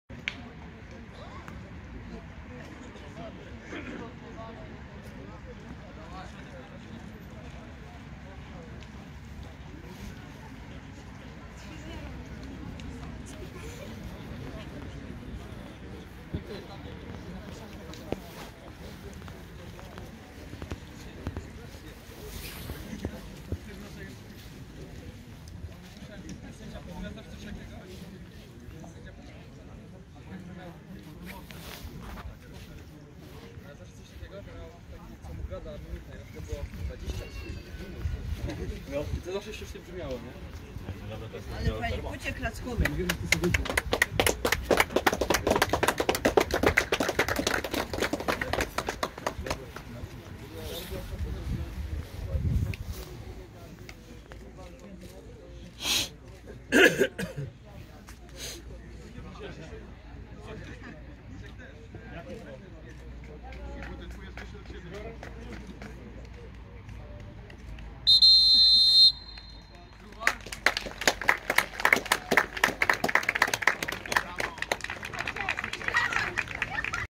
13. kolejka: Grunwald 4-3 Gwiazda Ruda Śląska
Zawodnicy grup młodzieżowych Grunwaldu wyprowadzają zawodników